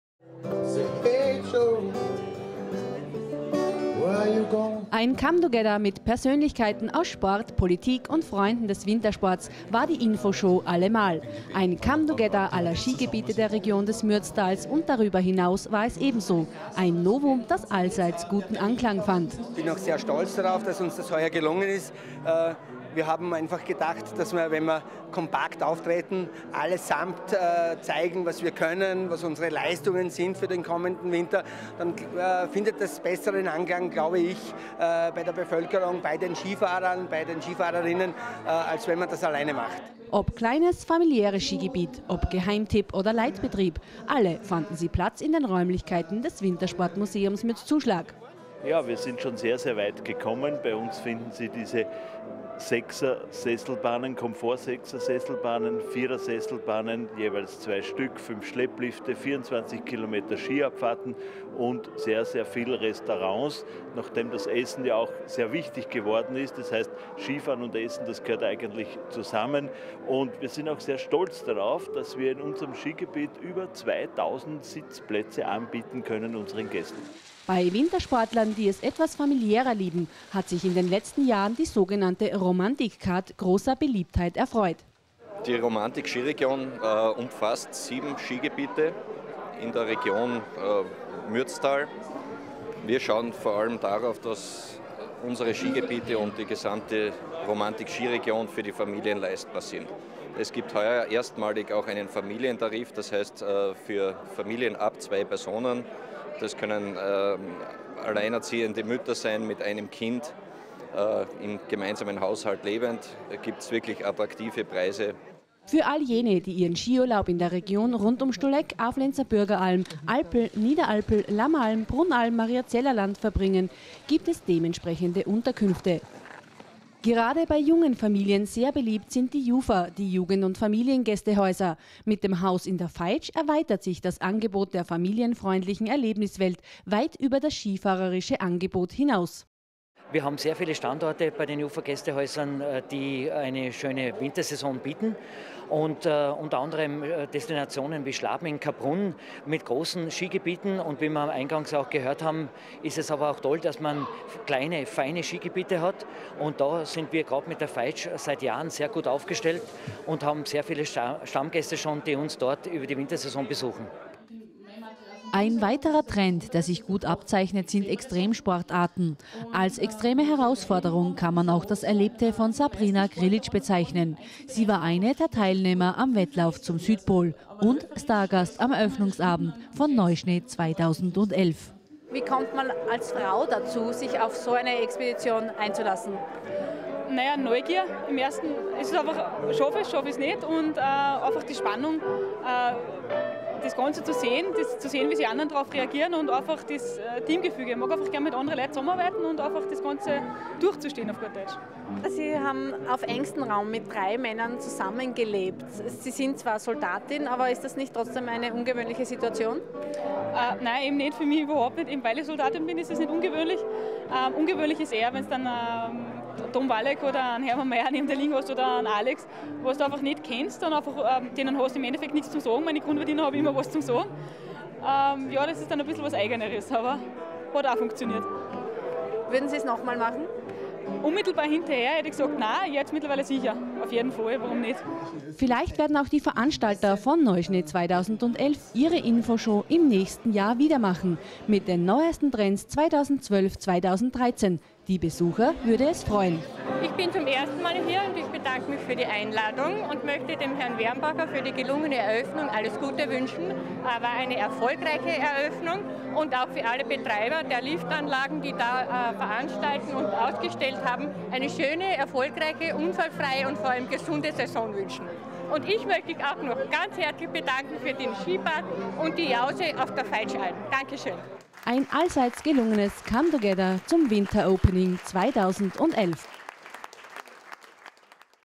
MEMA TV - Austellung "Neuschnee 201" in Mürzzuschlag KW 45
Im Rahmen der Mürzer Messe, die vor kurzem über die Bühne ging, fand im Wintersportmuseum die Ausstellung "Neuschnee 2011" statt -
Eine Infoshow rund um Wintersport und Tourismus. Zu entdecken gab´s dabei die innovativsten und aktuellsten Trends 2011.